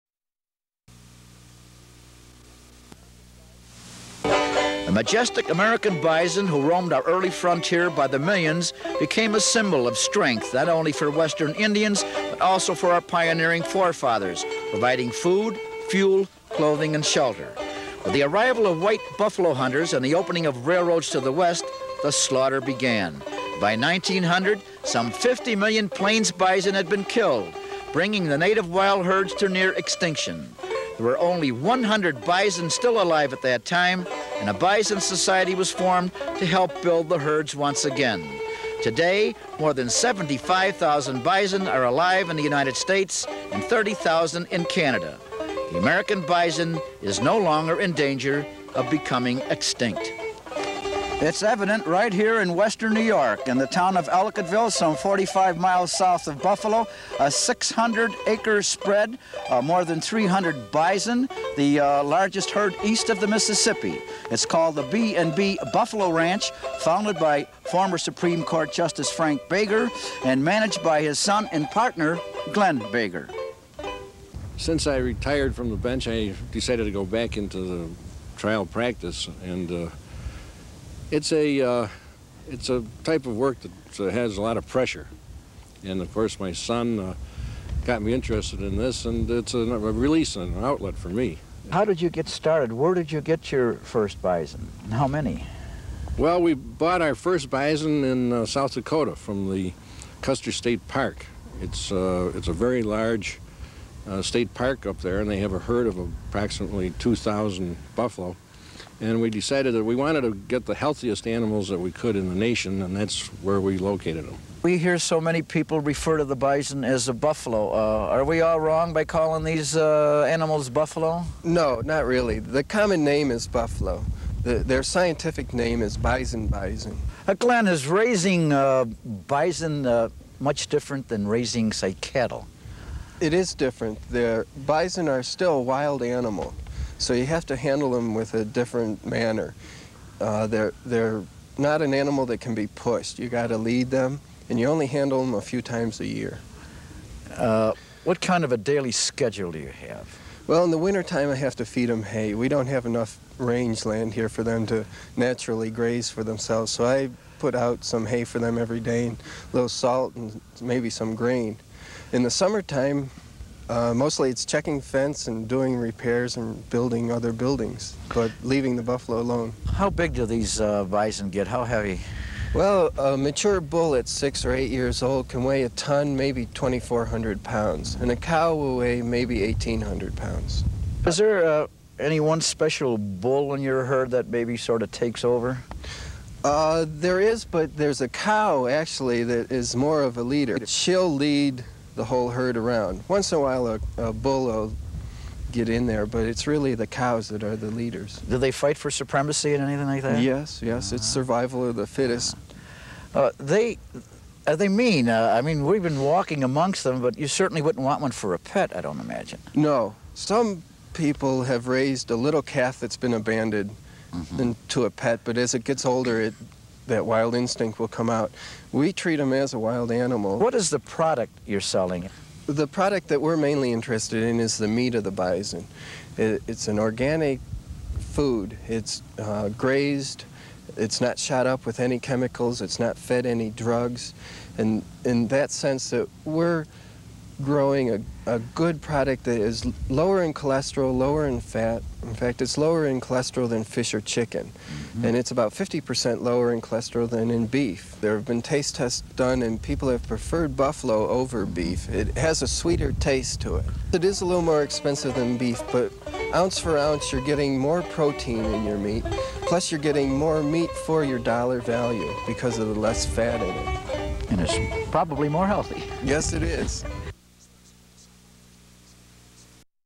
BISON HERD
BACK IN THE 1980'S HERDS OF BUFFALO WERE SEEN ROAMING THE  RANGES  SOUTH OF BUFFALO, NY  ...WITH FRAN LUCCA AT THE EDITOR'S DESK.